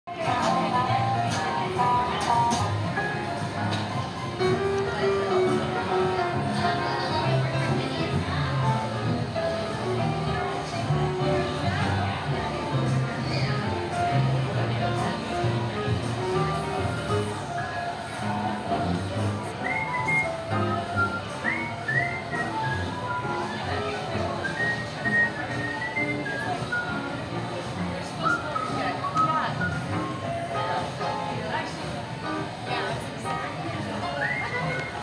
POP UP Disney a Mickey Celebration - Mickey and Minnie's Love at Downtown Disney
in Pop Up Disney a Mickey Celebration Mickey and Minnie's Love is the First Room you Enter with Mickey and Minnie's Relationship with a Photo op of Minnie's Bow and Other Things with Mickey's Love intest